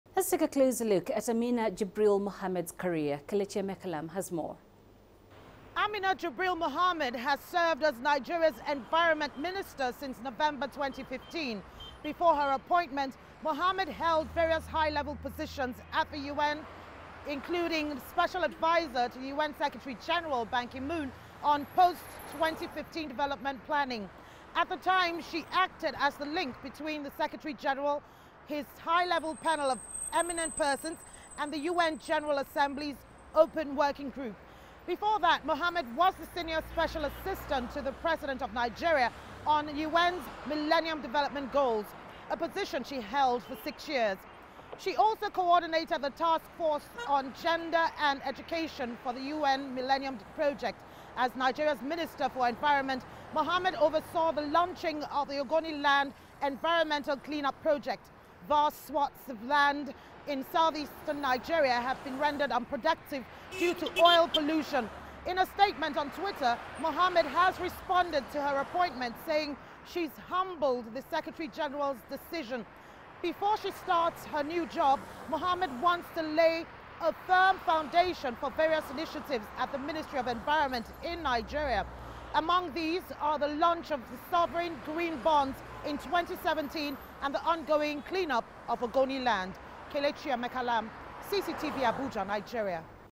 UN's Guterres appoints Nigeria's Amina Mohammed as his deputy
Incoming United Nations Secretary-General Antonio Guterres has appointed three women to top jobs in the organisation.  He has chosen Nigeria's environment minister Amina Mohammed as his deputy secretary-general. He's also appointed Brazilian diplomat Maria Luiza Ribeiro Viotti as chief of cabinet and Kyung-wha Kang of South Korea to a new position as special policy adviser. Guterres was sworn in on Monday as the ninth UN chief. He's pledged to ensure gender parity among senior leadership during his five-year term. Here is a closer look at Amina Jibril Mohammed's career. Kelechi Emekalam has more.